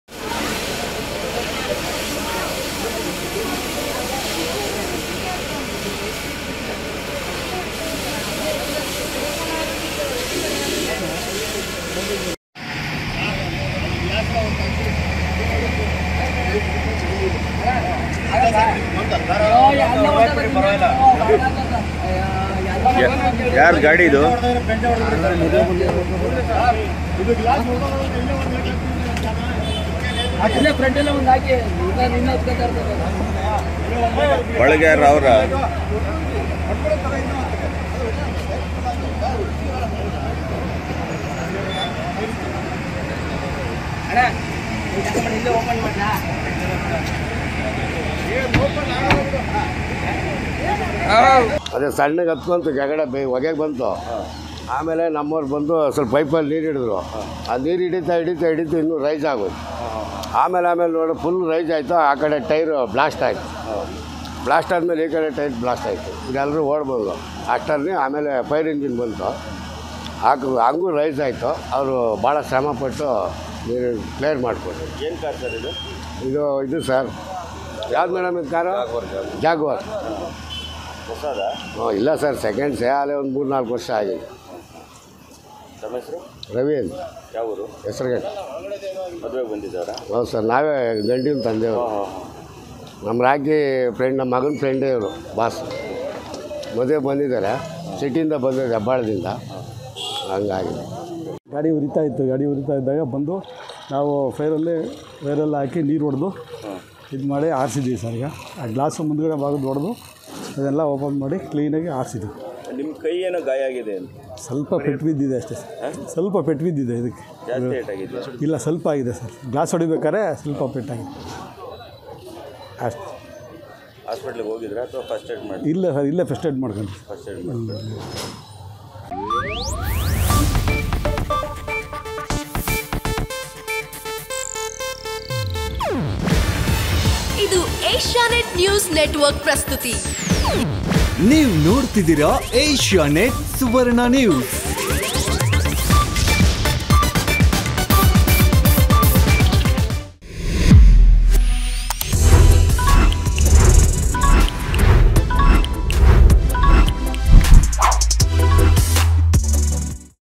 ಮದುಮಗನ ಕೋಟ್ಯಾಂತರ ರೂ ಮೌಲ್ಯದ ಜಾಗ್ವಾರ್ ಕಾರಿಗೆ ಬೆಂಕಿ | Tumakuru News | Kannada News | Suvarna News
ಕೋಟ್ಯಾಂತರ ರೂ ಮೌಲ್ಯದ ಜಾಗ್ವಾರ್ ಕಾರಿಗೆ ಬೆಂಕಿ.ತುಮಕೂರು ಜಿಲ್ಲೆಯ ಕುಣಿಗಲ್ ಪಟ್ಟಣದಲ್ಲಿ ಘಟನೆ. ಮದುವೆಗೆಂದು ಬಂದಿದ್ದ ವರನ ಕಾರಿಗೆ ಬೆಂಕಿ. ಕುಣಿಗಲ್ ನ ನಕ್ಷತ್ರ ಪ್ಯಾಲೇಸ್ ಕಲ್ಯಾಣ ಮಂಟಪಕ್ಕೆ ಬಂದಿದ್ದ ಕಾರು. ಶಾರ್ಟ್ ಸರ್ಕ್ಯೂಟ್ ನಿಂದ ಕಾರಿನಲ್ಲಿ ಕಾಣಿಸಿಕೊಂಡ ಬೆಂಕಿ. ಅಗ್ನಿ ಶಾಮಕದಳದಿಂದ ಬೆಂಕಿ‌ನಂದಿಸುವ ಕಾರ್ಯ. ಕುಣಿಗಲ್ ಪೊಲೀಸ್ ಠಾಣೆ ವ್ಯಾಪ್ತಿಯಲ್ಲಿ ಘಟನೆ.